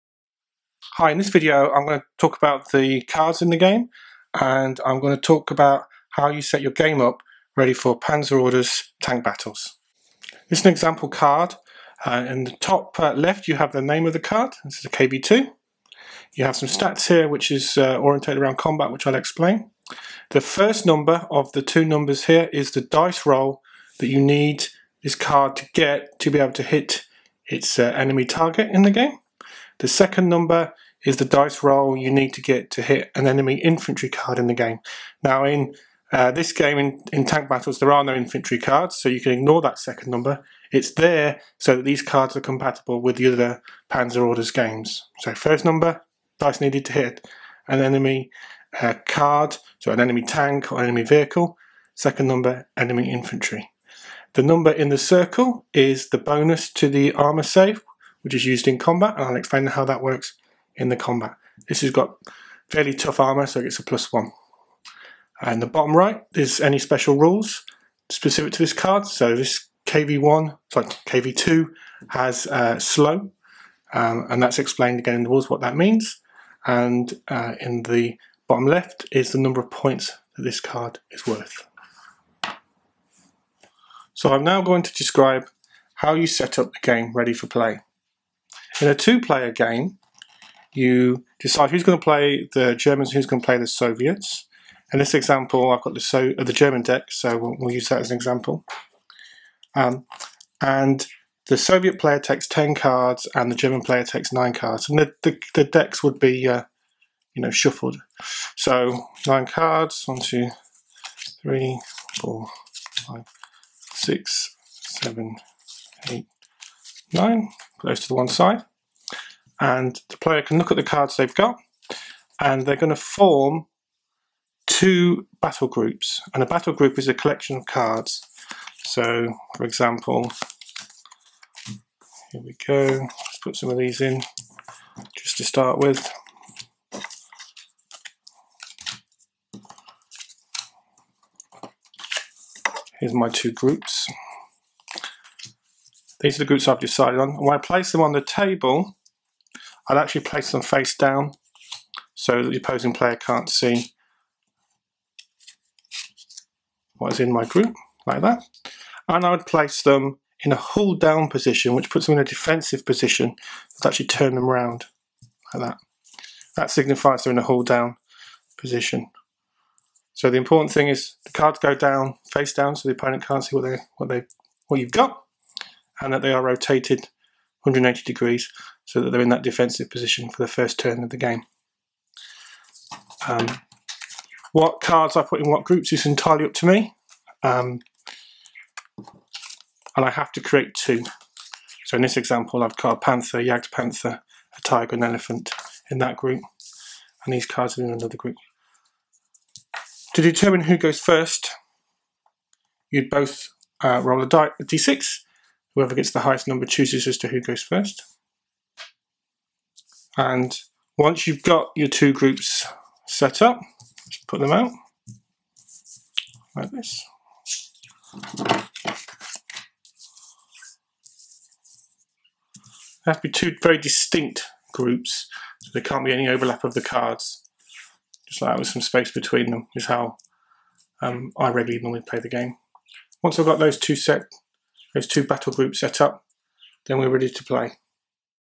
Panzer Orders Tanks Battles - Card Description and Game Setup
Description of a card from Panzer Orders Tank Battles and how to setup your two player game.